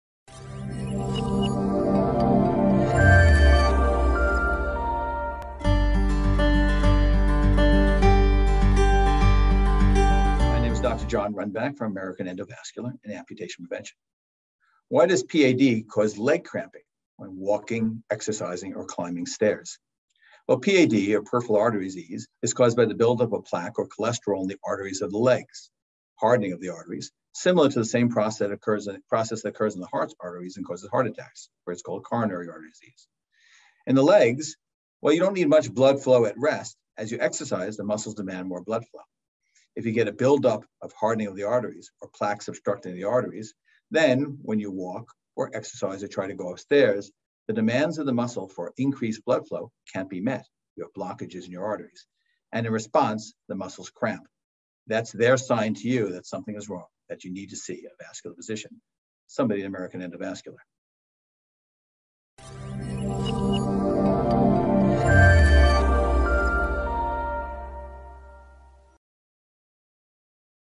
Why does PAD cause muscle cramping in the legs while walking, exercising, or climbing stairs?
PAD or Peripheral Arterial Disease is caused by the build-up of plaque or cholesterol in the arteries of the legs, the hardening of the arteries, similar to the same process that occurs in the heart's arteries that causes heart attacks where it's called Coronary Heart Disease. In the legs, you don't need much blood flow at rest, but as you exercise, the muscles demand more blood flow. When there is a build-up of plaque. Obstructing the arteries, the increased demand for blood flow from leg muscles when you walk, exercise, or climb up stairs can't be met. This is due to arterial blockages and the muscles cramping, both signs indicating that something is wrong. That is the time to see a vascular physician.